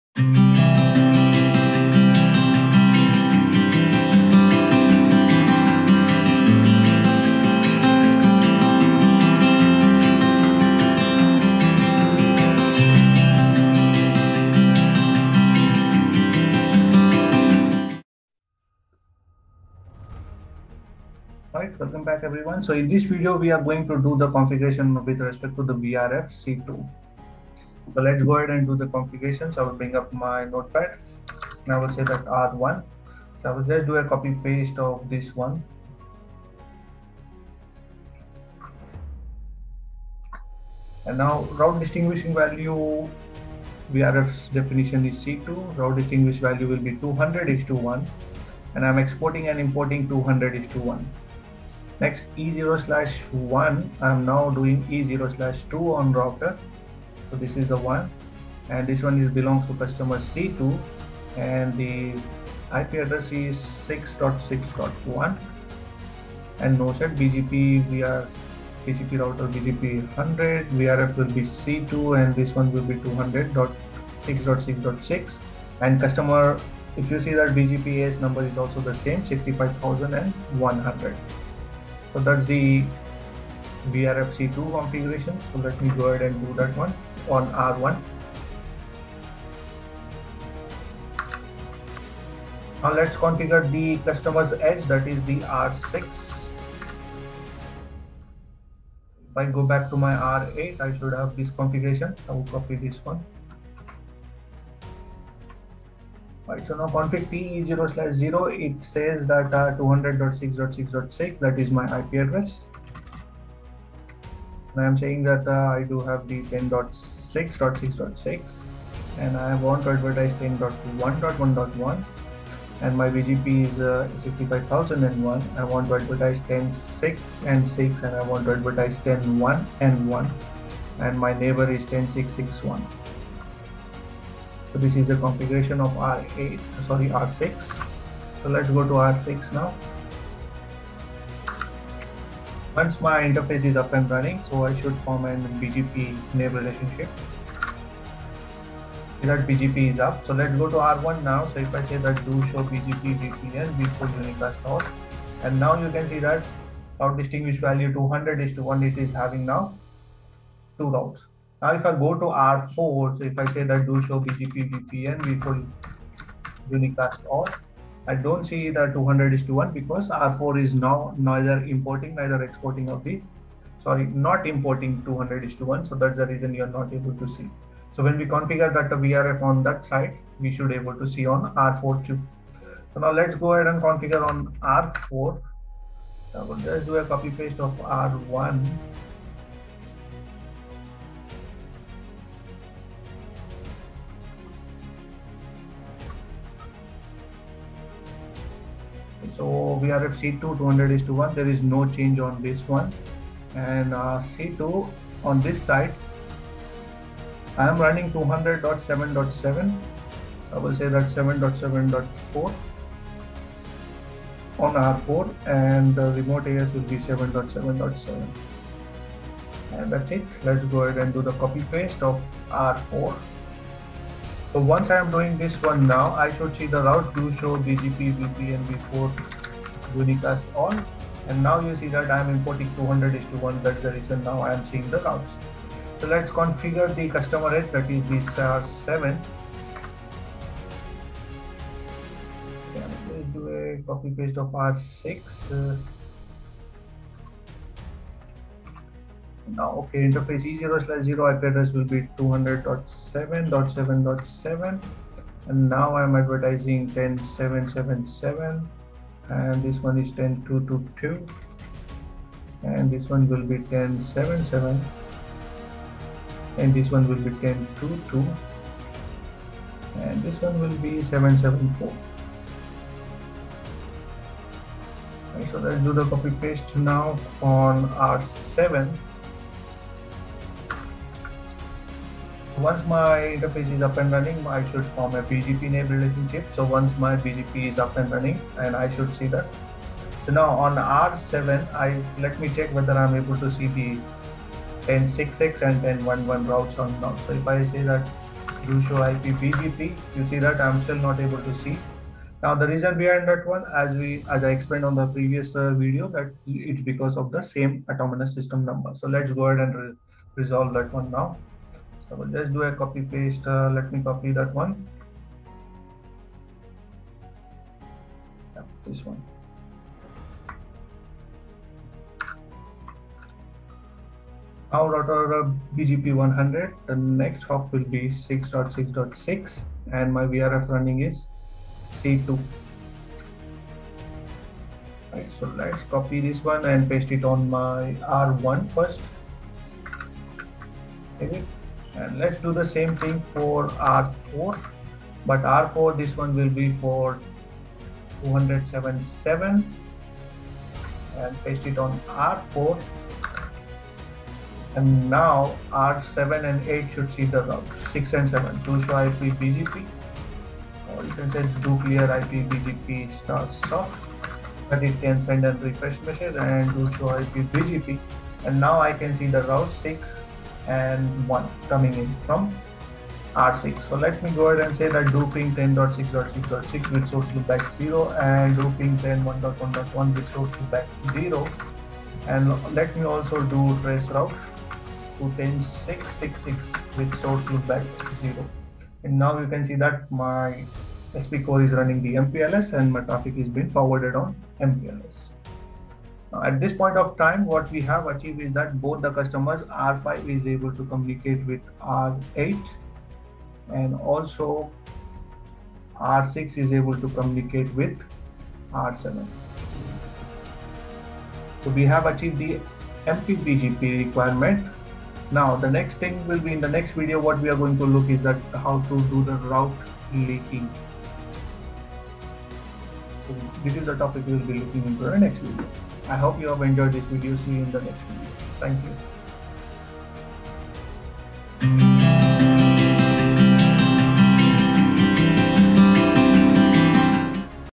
MPLS MP-BGP VRF CONFIG 2
Hi all in this video we are going to configure the VRF's on R1 and R4 so that they can share the customer routes.

CANVA